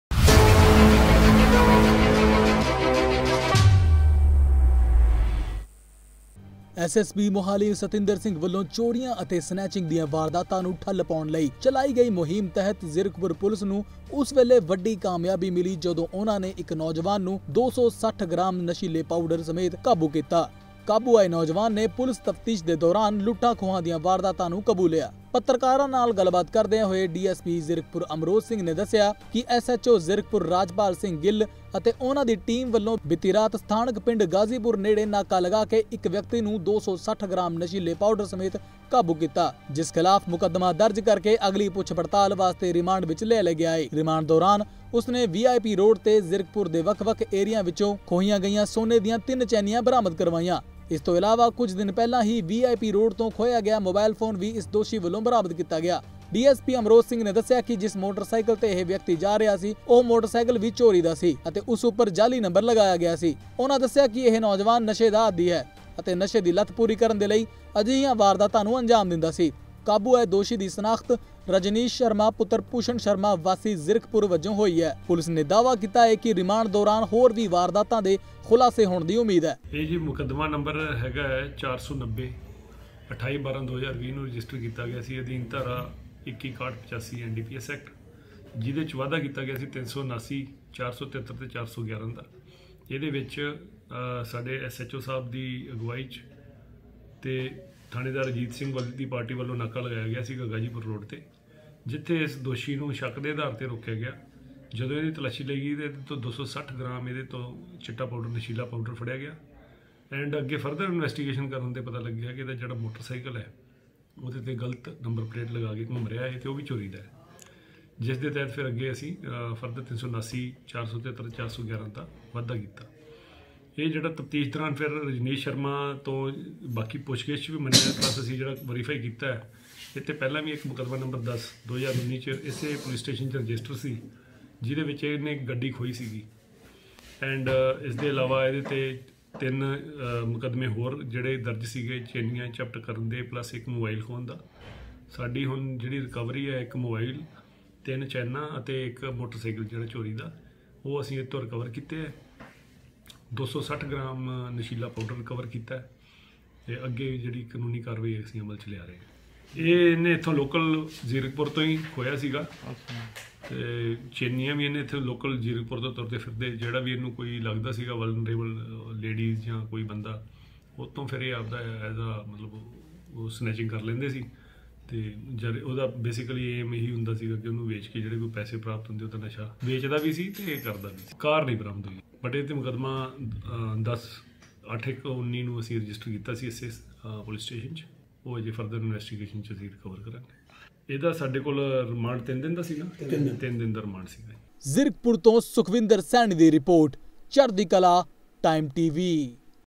260 ਗ੍ਰਾਮ ਨਸ਼ੀਲੇ ਪਾਊਡਰ ਸਮੇਤ ਇੱਕ ਨੌਜਵਾਨ ਕਾਬੂ
260 ਗ੍ਰਾਮ ਨਸ਼ੀਲੇ ਪਾਊਡਰ ਸਮੇਤ ਇੱਕ ਨੌਜਵਾਨ ਕਾਬੂ